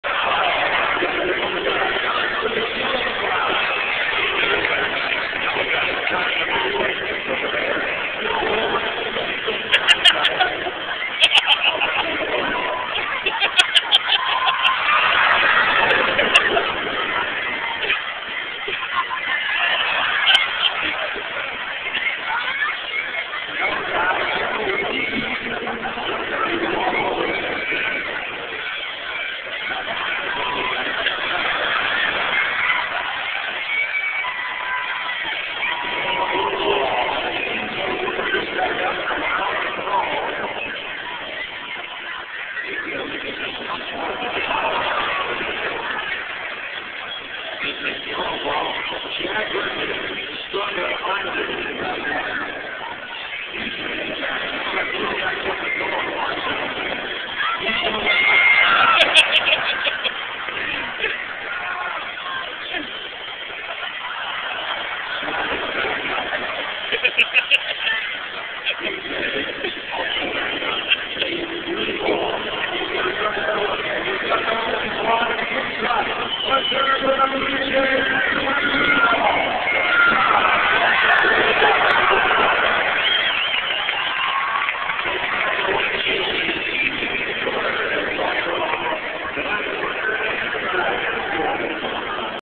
Chad and PJ Hamster Ball Race
PJ wins the 1st intermission entertainment.  He then went on to be signed by the NHBL (National Hamster Ball League)